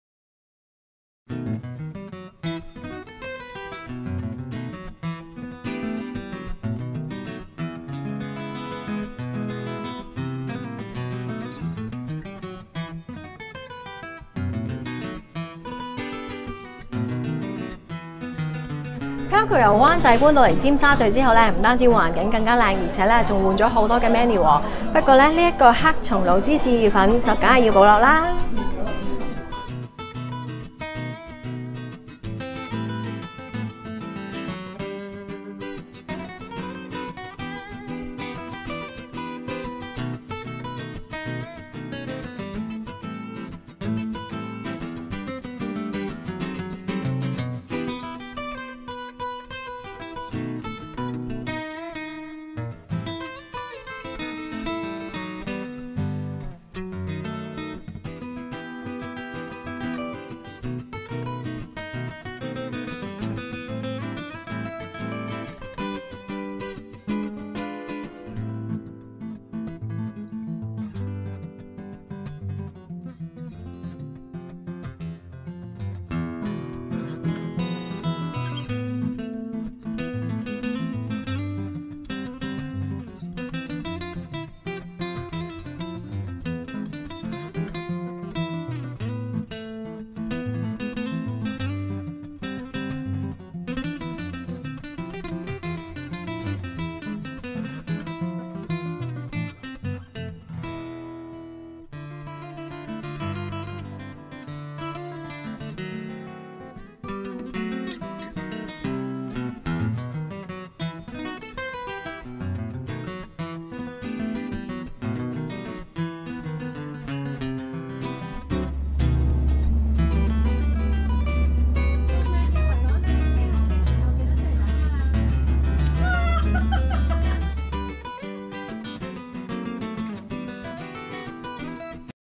名廚主理＋名人明星熱捧！維港靚景Palco Ristorante
【名廚主理＋名人明星熱捧！
維港靚景Palco Ristorante】

大家還記得我之前介紹過西九文化區的Pano嗎？由名廚Ken Lau主理的十道菜高質晚餐，至今仍叫我難忘。

其實Ken Sir在灣仔一直有經營另一餐廳Palco Ristorante，直至今年才搬到尖沙咀海運OTE，不但有無敵海景環境更靚，餐牌還改頭換面，換了不少新菜式，不過招牌菜「Spaghetti with Parmesan Cheese and Black Truffle Paste」就當然要保留，Ken Sir即席在客人面前烹調，濃郁的芝士及黑松露香氣在餐廳的空氣中飄散，沒有多少食客可以抵擋得住這誘惑！

大家看影片就知道很多美食都是打卡級，看似番茄的芝士，紫色的紅菜頭意粉，還有像隻小船的海膽配墨魚脆片，全部色香味全，難怪Ken Sir儲了大批名人明星客，不是說笑，這天鄰桌就坐著金牌經理人黃柏高 Paco。

吃完這一餐，真的很滿足，我真心想說一聲：多謝Paco！吖不是，是多謝Palco！

[Palco Ristorante]
地址：尖沙咀廣東道3-27號海港城海運大廈4樓403-404號舖
營業時間：每天12:00 - 15:00，18:00 - 23:00